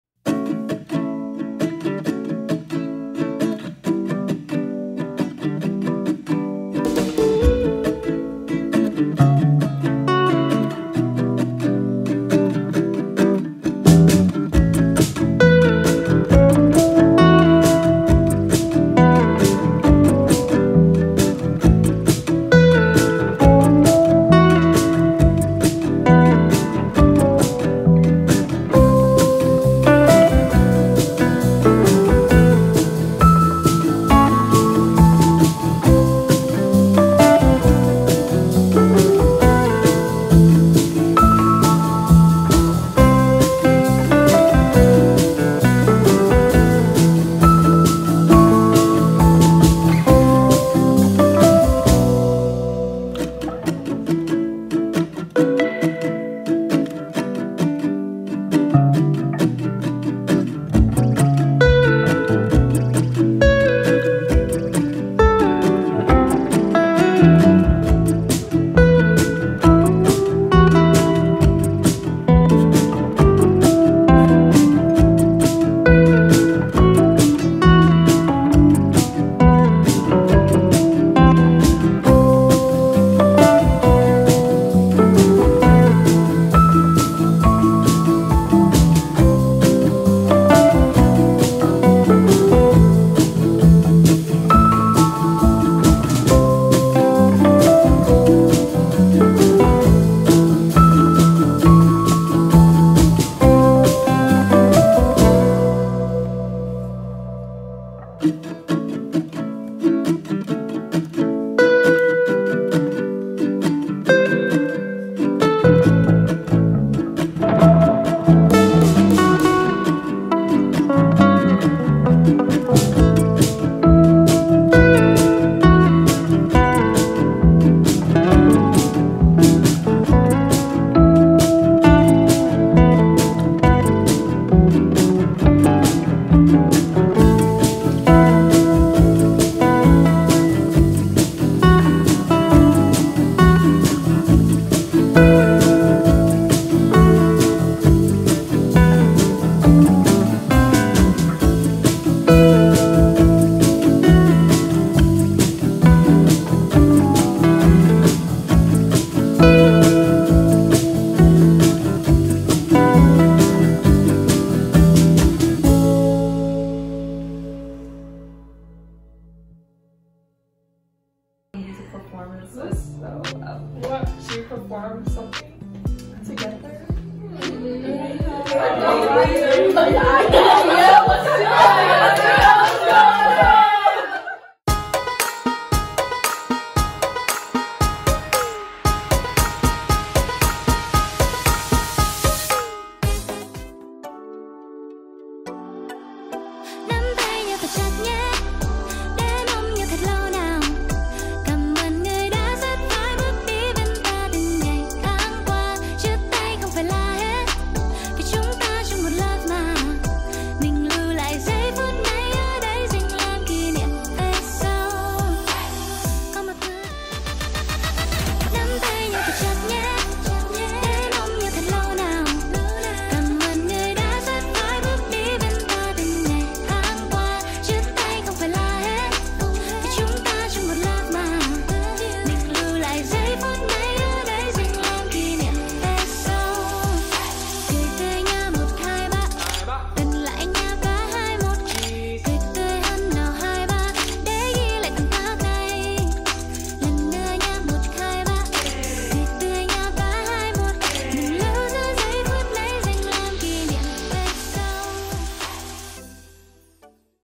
2020-21 End of the Year Video
To commemorate the amazing year we have had with our general members, we have gathered all our memories we have made together and put it in our End-of-the-Year video! Thanks for all your support this year -- we hope to see you again next year :)

Edited By: Lucy Nguyen
*Music does not belong to us*